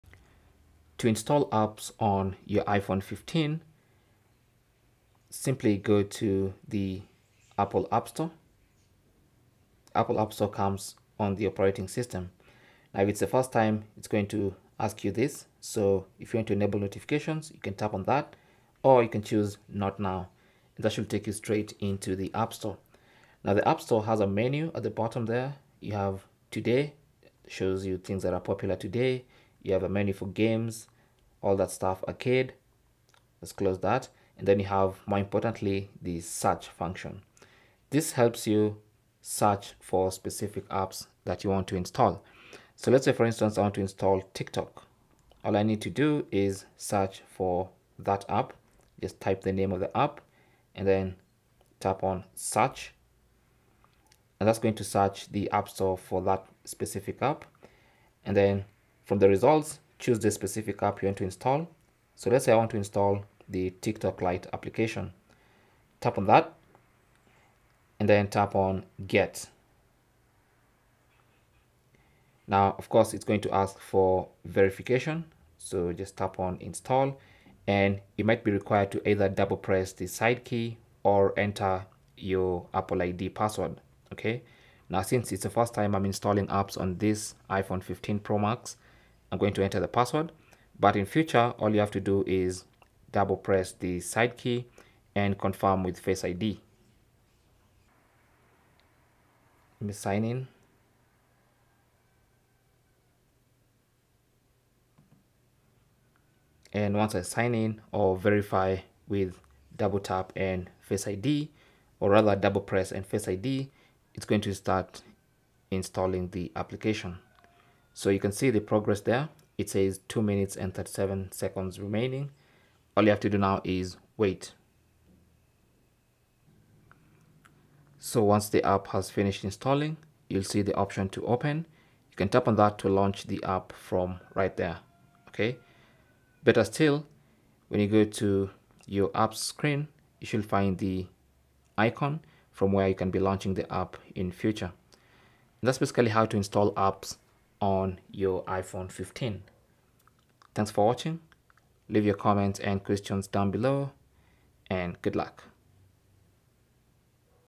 How To Install Apps On iPhone 15 & iPhone 15 Pro
Learn How To Install Apps On iPhone 15 & iPhone 15 Pro. Welcome to our step-by-step guide on how to install apps on the latest iPhone 15 and iPhone 15 Pro.

No more confusion, just a seamless app installation process!

Timestamps:
0:00 - Opening App Store
0:36 - Using Search Function
1:06 - Installing An App
2:07 - Launching The App

Cell Phone Tripod Adapter